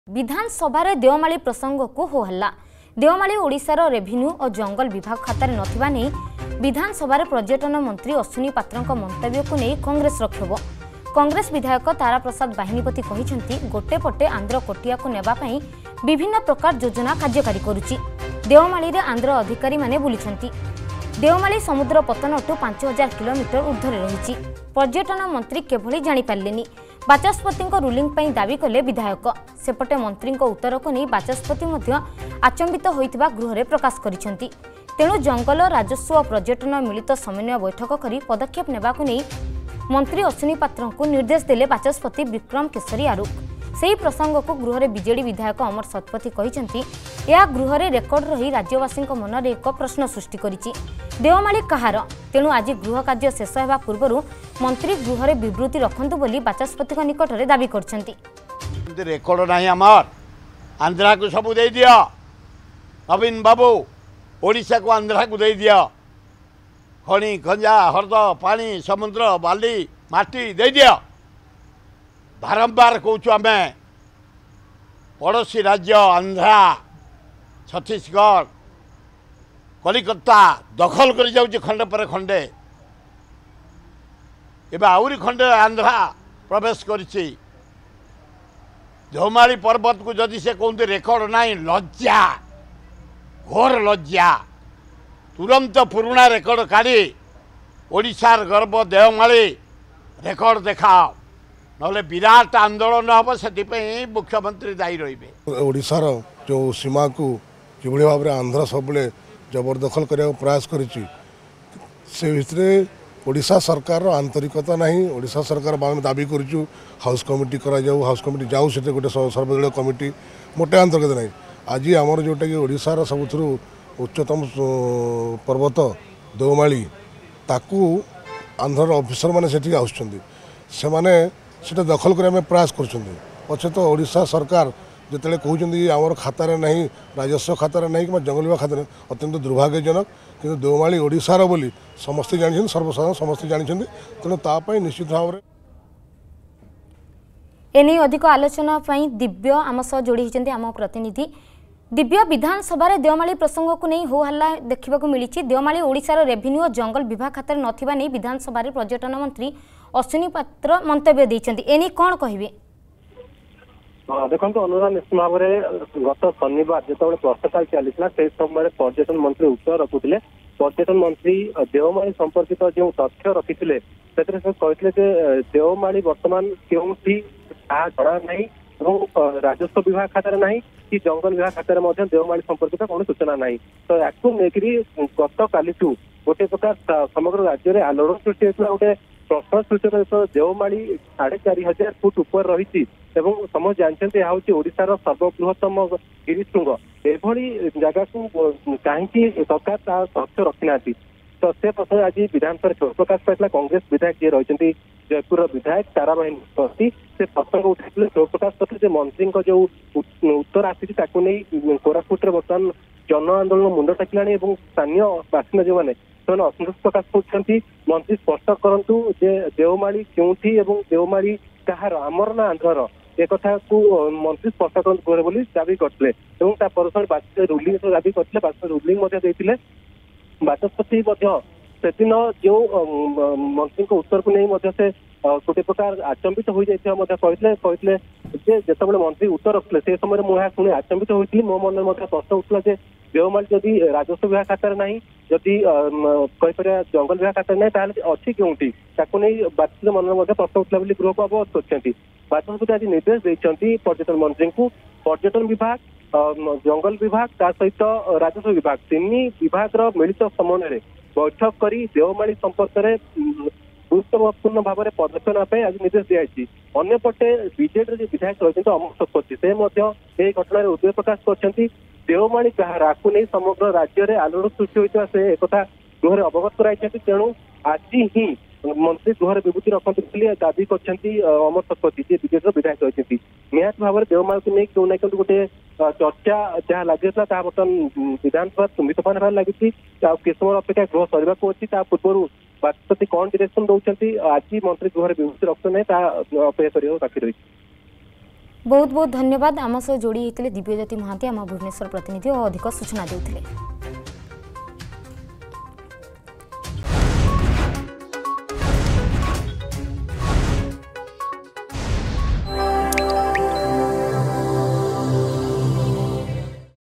ବିଧାନସଭାରେ ଦେଓମାଳି ପ୍ରସଙ୍ଗକୁ ହୋହଲ୍ଲା
ଦେଓମାଳି ଓଡ଼ିଶାର ରେଭିନ୍ୟୁ ଓ ଜଙ୍ଗଲ ବିଭାଗ ଖାତାରେ ନଥିବା ନେଇ ବିଧାନସଭାରେ ପର୍ଯ୍ୟଟନ ମନ୍ତ୍ରୀ ଅଶ୍ବିନି ପାତ୍ରଙ୍କ ମନ୍ତବ୍ୟକୁ ନେଇ କଂଗ୍ରେସର କ୍ଷୋଭ । କଂଗ୍ରେସ ବିଧାୟକ ତାରାପ୍ରସାଦ ବାହିନୀପତି କହିଛନ୍ତି, ଗୋଟେ ପଟେ ଆନ୍ଧ୍ର କୋଟିଆକୁ ନେବା ପାଇଁ ବିଭିନ୍ନ ପ୍ରକାର ଯୋଜନା କାର୍ଯ୍ୟକାରୀ କରୁଛି । ଦେଓମାଳିରେ ଆନ୍ଧ୍ର ଆଧିକରି ମାନେ ବୁଲୁଛନ୍ତି ।